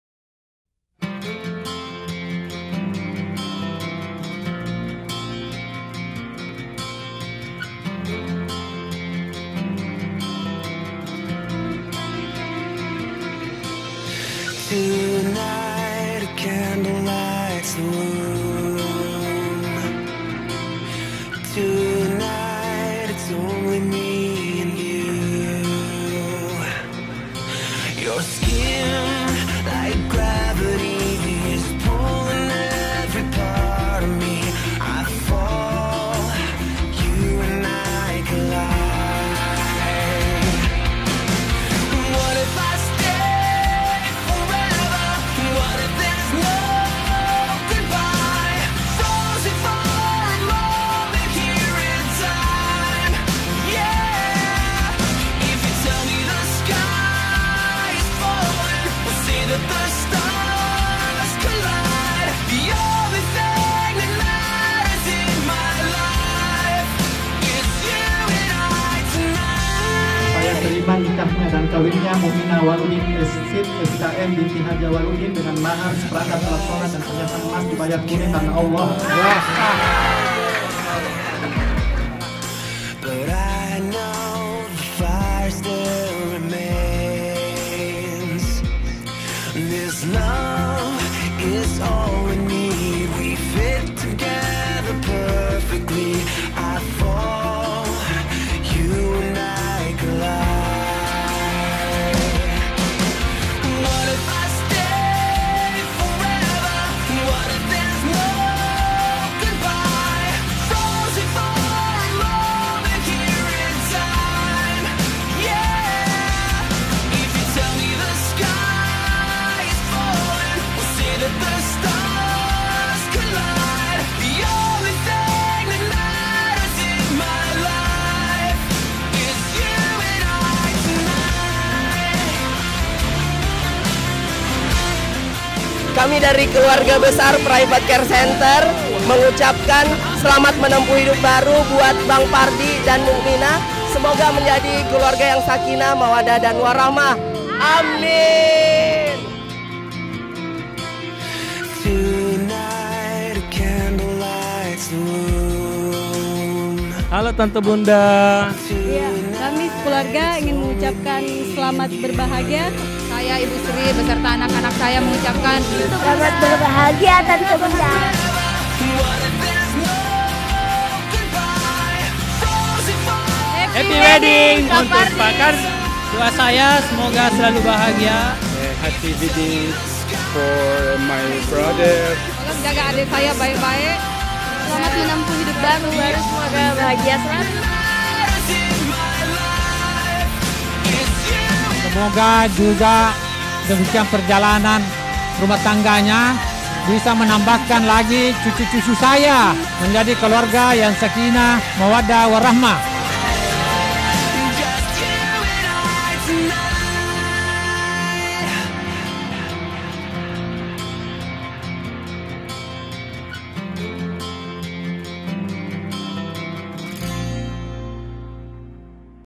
Pardi & Ina | Video Pernikahan Bugis Makassar (Wedding Clip Video Cinematic)
Video Pernikahan, Wedding Clip, Best Wedding Video, Wedding Cinematic, Wedding Trailer for Supardi Said & Mu’minah Awaluddin Wedding Ceremony (Pernikahan Muslim Adat Bugis Makassar) di Makassar. Video by Mattuju Indonesia Indonesia Wedding Photographer & Videographer based in Makassar, South Sulawesi. Indonesia.

Edit by: Wahyu Ashar Ariyadi